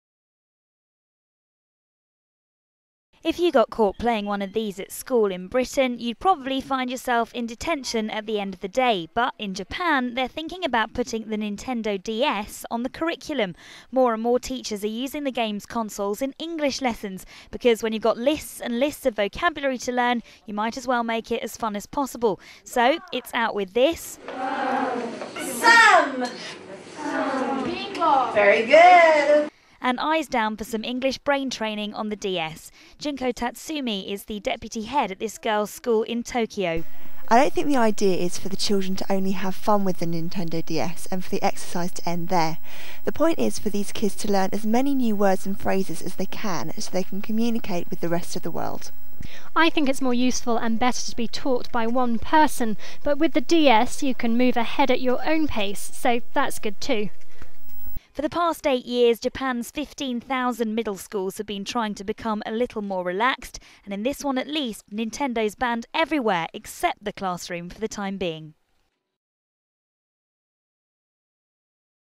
Nintendo lessons
Children in Japan are being encouraged to ditch their textbooks and get their Nintendos out instead. Test